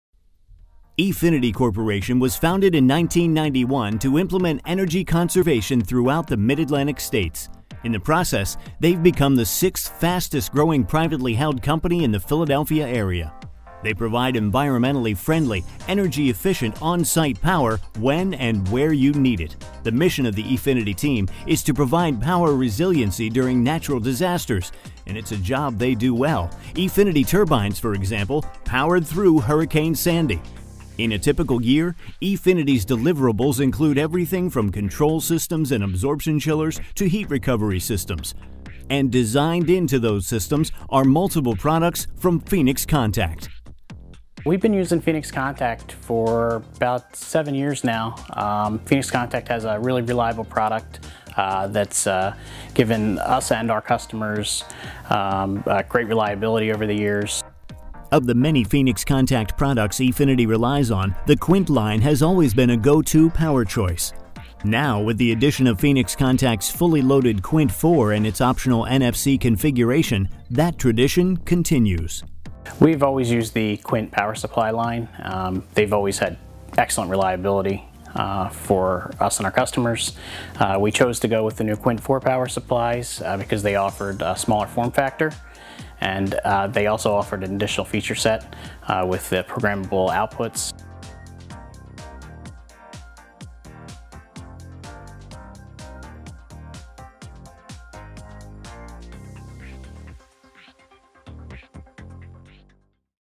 E-Finity’s go-to power choice is the fully-loaded QUINT 4
One of the most uncontrollable powers in the world is Mother Nature. One of the most controllable powers in the world is properly supplied recovery systems. E-Finity, an environmentally friendly, energy-efficient, on-site power provider for natural disasters, uses turbines and other control systems to aid those in need. When lives are on the line, they depend upon Phoenix Contact to ensure that their equipment does the job. Of the many Phoenix Contact products E-Finity relies on, the QUINT line has always been their go-to power choice. Now, with the addition of the fully-loaded QUINT 4 and its optional NFC configuration, that tradition continues.

Transcript:  E-Finity Corporation was founded in 1991 to implement energy conservation throughout the mid-Atlantic states. In the process, they've become the sixth fastest growing privately held company in the Philadelphia area. They provide environmentally friendly, energy efficient on-site power when and where you need it. The mission of the E-Finity team is to provide power resiliency during natural disasters and it's a job they do well. E-Finity turbines, for example, powered through Hurricane Sandy. In a typical year, E-Finity's deliverables include everything from control systems and absorption chillers to heat recovery systems. And designed into those systems are multiple products from Phoenix Contact.
Phoenix Contact has a really reliable product that's given us and our customers great reliability over the years.
Of the many Phoenix Contact products E-Finity relies on, the QUINT line has always been a go-to power choice. Now with the addition of Phoenix Contact's fully loaded QUINT 4 and its optional NFC configuration, that tradition continues.
We've always used the QUINT power supply line. They've always had excellent reliability for us and our customers. We chose to go with the new QUINT 4 power supplies because they offered a smaller form factor and they also offered additional feature set with the programmable outputs.

We bring information, how to’s, tutorials, and case studies on machine learning, industrial internet, technology, software, STEM, and more!